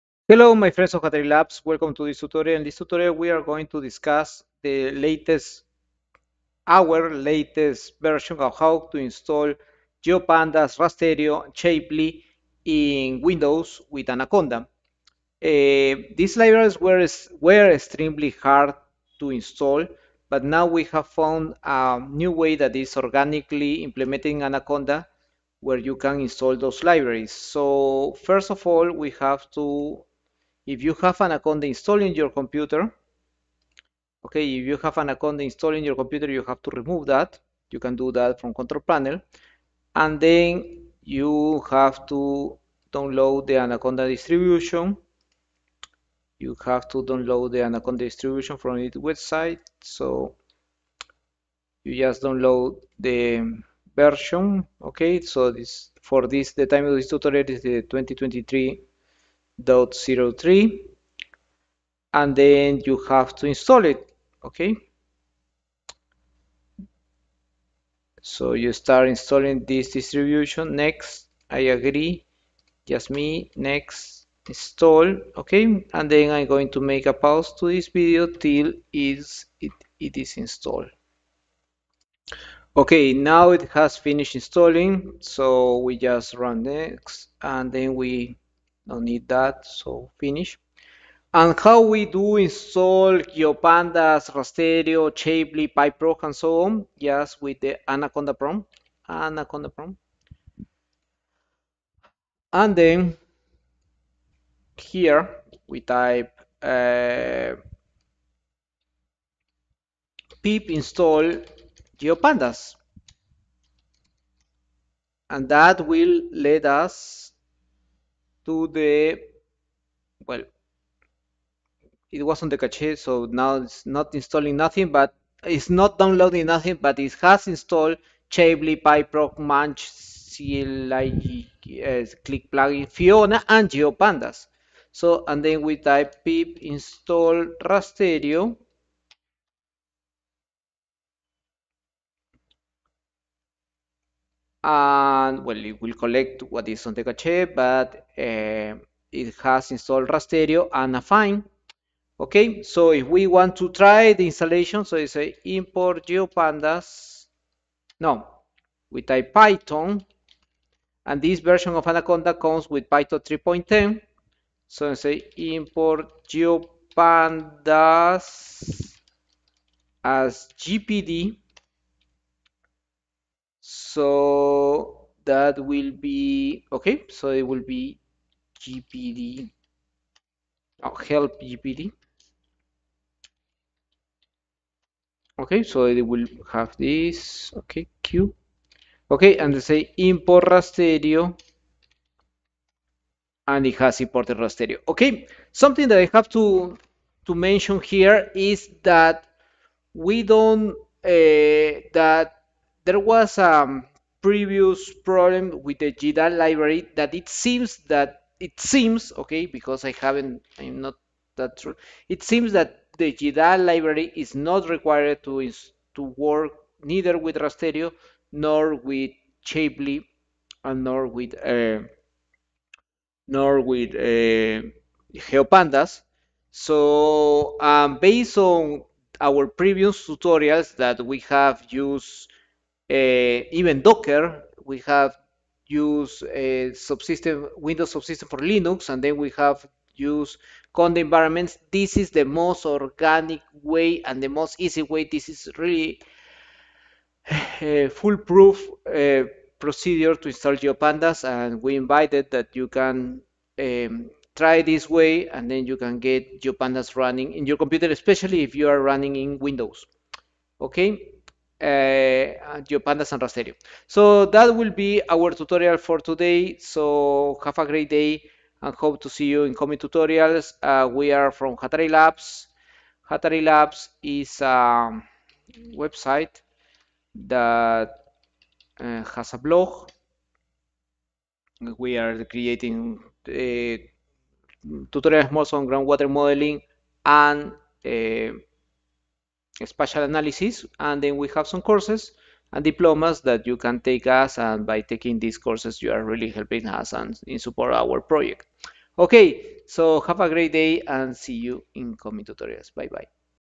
How to install (easy way) Python, Geopandas and Rasterio in Windows - Tutorial
Geopandas is an amazing library for spatial analysis since in combines the spatial tools from Shapely and Fiona with the versatily of Pandas Dataframes. Rasterio is one of the most complete libraries to deal with raster files in Python.
We are aware that most geoscientists, water resources specialists and related professionals work on Windows, therefore we are always in the search of new ways to get Python working with all its geospatial capabilities in every computer.
Based on the new Anaconda releases now we can install Geopandas and Rasterio in a easy way.